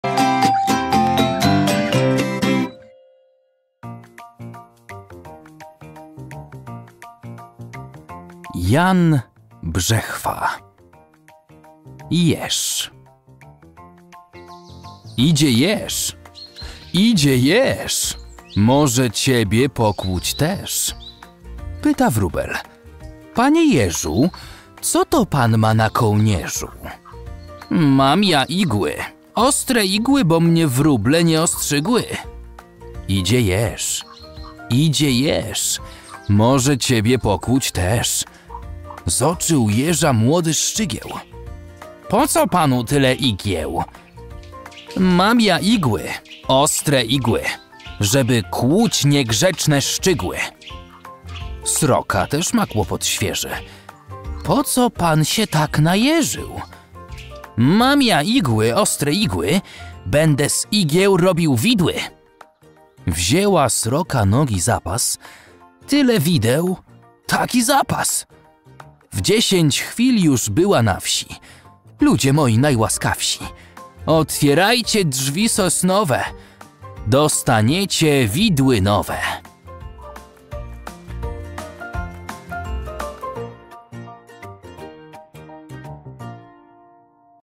🦔 Jan Brzechwa – Jeż | Bajki Polskie 🎵
🎈Opowiadamy małe przygody dla wielkich odkrywców!

Idzie jeż, idzie jeż,
Może ciebie pokłuć też!

Pyta wróbel: „Panie jeżu,
Co to pan ma na kołnierzu?”

„Mam ja igły, ostre igły,
Bo mnie wróble nie ostrzygły!”

Zoczył jeża młody szczygieł:
„Po co panu tyle igieł?”

Żeby kłuć niegrzeczne szczygły!”

Sroka też ma kłopot świeży:
„Po co pan się tak najeżył?”

Będę z igieł robił widły!”

Wzięła sroka nogi za pas:
„Tyle wideł! Taki zapas!”

W dziesięć chwil już była na wsi:
„Ludzie moi najłaskawsi,
Otwierajcie drzwi sosnowe,
Dostaniecie widły nowe!”

🧡Tu posłuchasz naszych bajek:
→ Google Podcasts
→ Apple Podcasts

Tytuł: Jan Brzechwa – Jeż
Opowiada: Artur Zajkowski